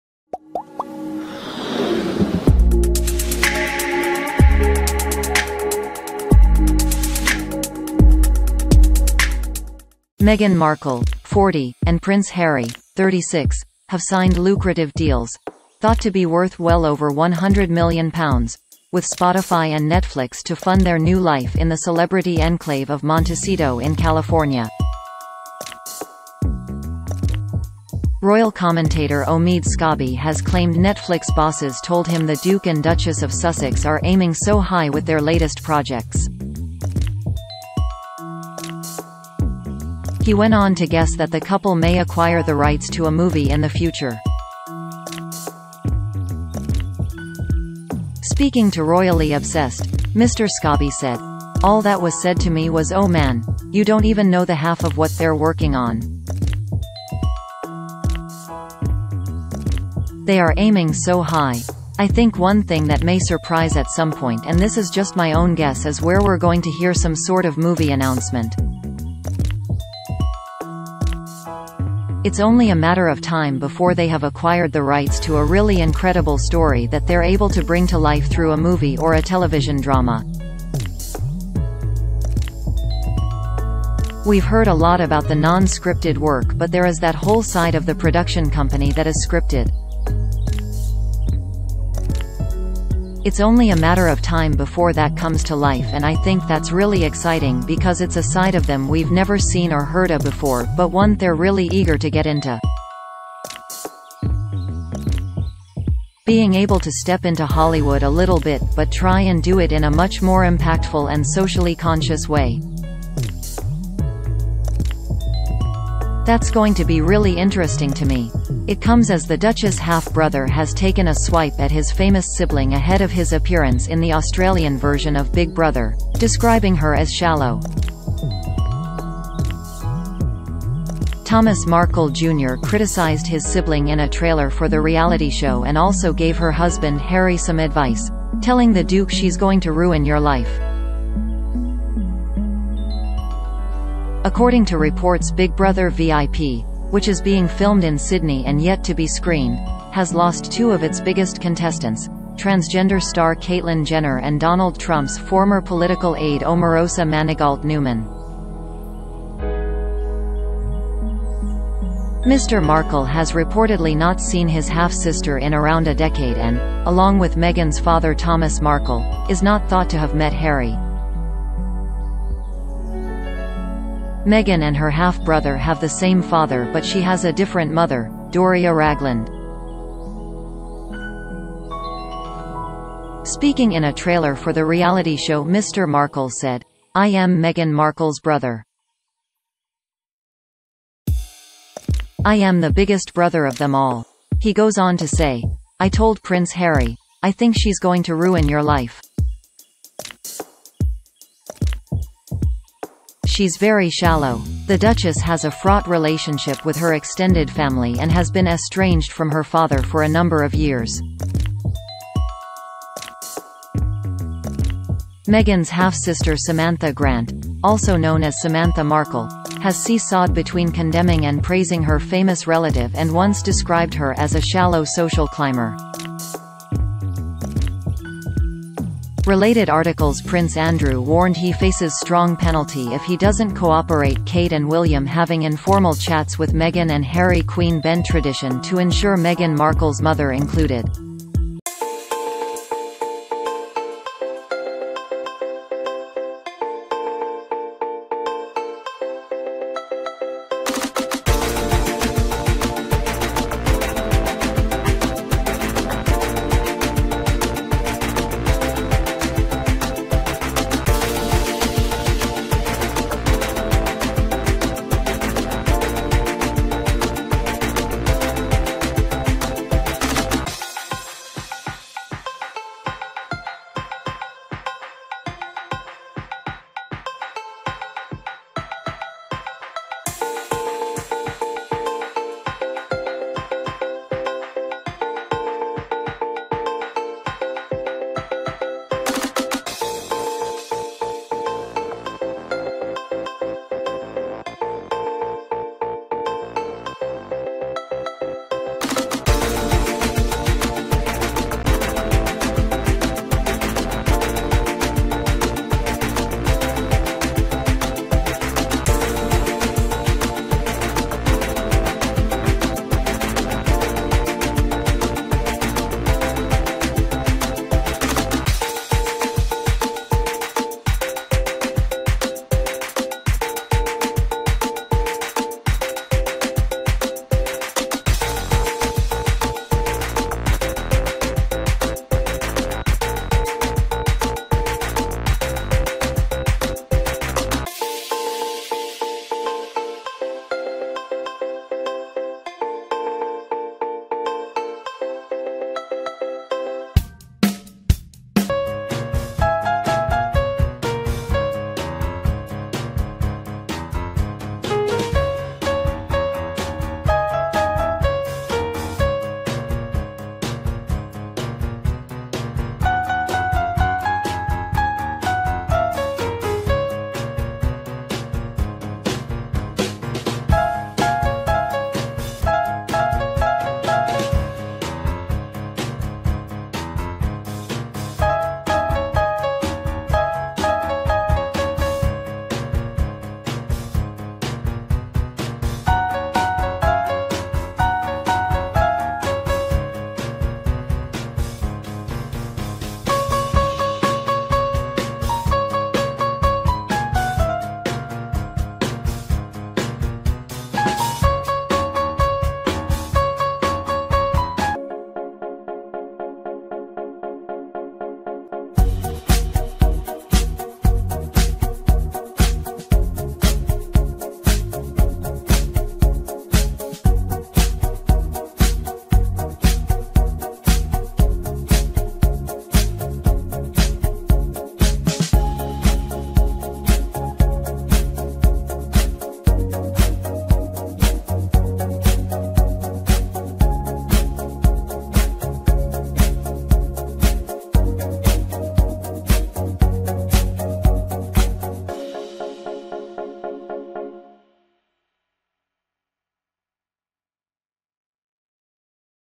Meghan Markle 'stepping back into Hollywood' - Duchess 'aiming so high' in new projects
Meghan Markle, 40, and Prince Harry, 36, have signed lucrative deals - thought to be worth well over £100 million - with Spotify and Netflix to fund their new life in the celebrity enclave of Montecito in California. Royal commentator Omid Scobie has claimed Netflix bosses told him the Duke and Duchess of Sussex are "aiming so high" with their latest projects. He went on to guess that the couple may acquire the rights to a movie in the future. Speaking to Royally Obsessed, Mr Scobie said: "All that was said to me was 'oh man, you don't even know the half of what they're working on. They are aiming so high'. "I think one thing that may surprise at some point and this is just my own guess is   ...